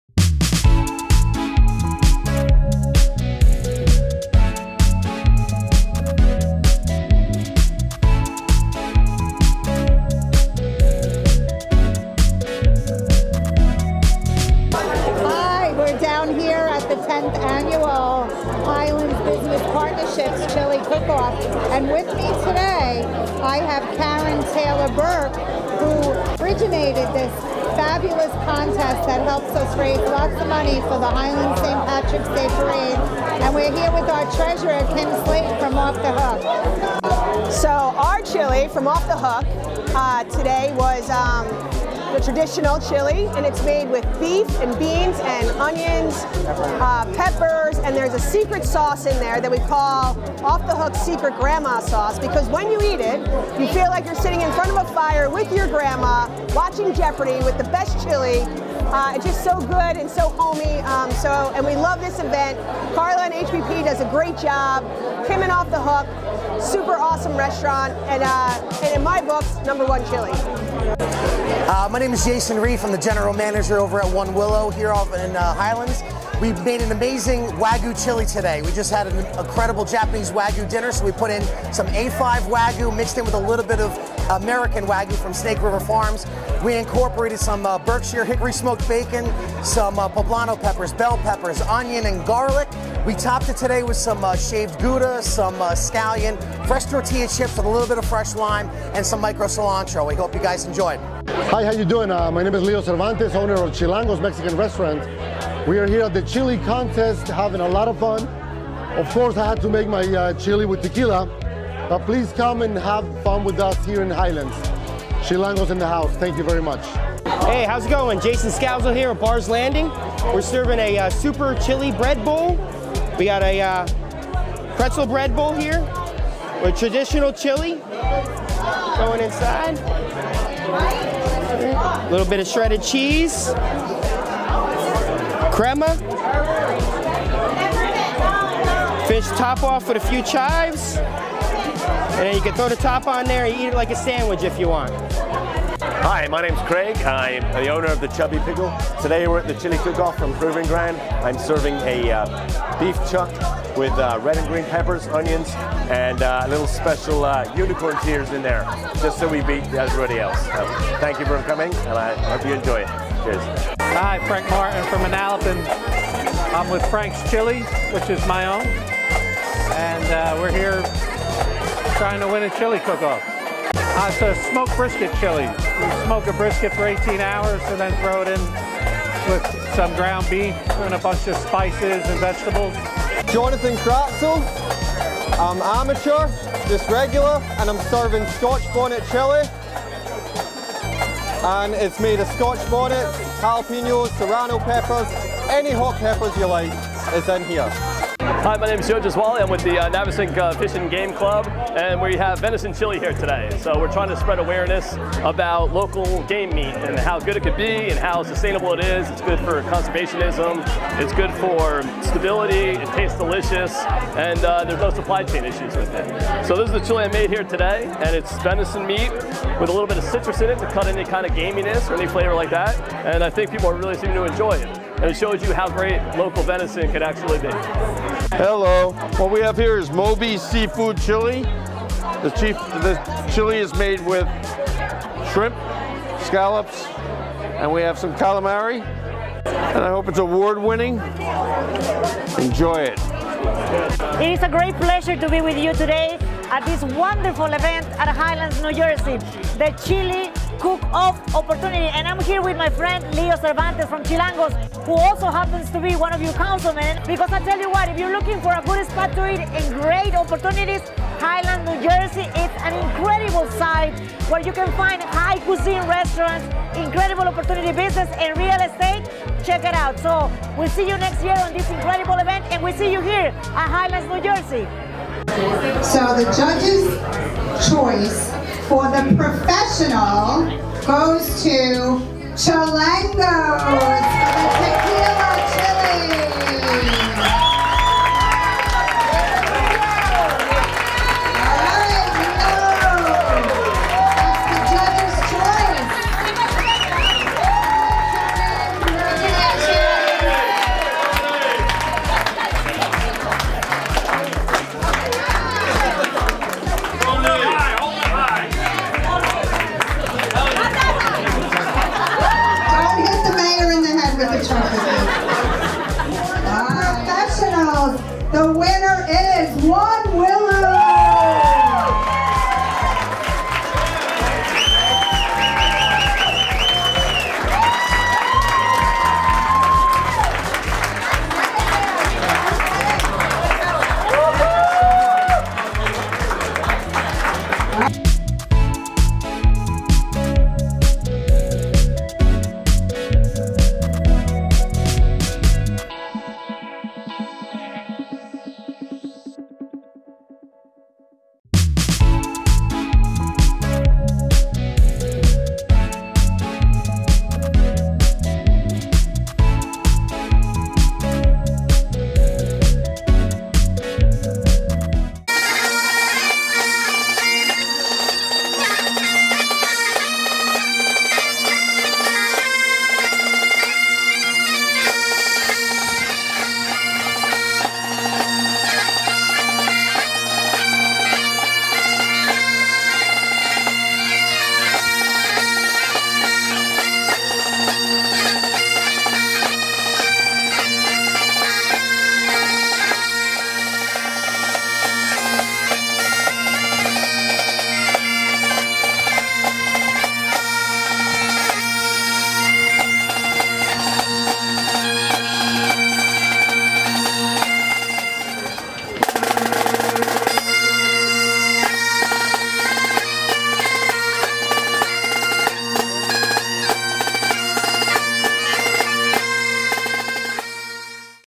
Highlands Chili Cook Off 2022
The Highlands Business Partnership’s Chili Cook Off fundraiser. There were two Chili Cook Off contests. The “Professional” Cook Off (commercial) and the “Amateur” Cook Off (residents). Once the chili lovers tasted all contestants’ chili, they voted for their favorite and drop their ballot in the ballot box.